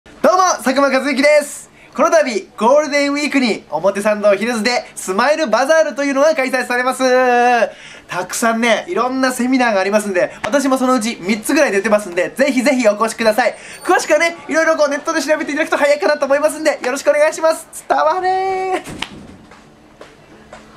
スマイルバザールさっくんコメント
5月6日（金）19：00より表参道ヒルズで開催中のSmile Bazarセミナー「スマホBATTLE 」に出演する佐久間一行から告知コメントが届きました！
『スマホBATTLE』
【期間】5月6日（金）19：00～20：00
【場所】表参道ヒルズ本館B3F ステージオー
【出演】博多華丸・大吉【MC】、佐久間一行、ボン溝黒（カナリア）、ブロードキャスト、向清太朗（天津）、大西ライオン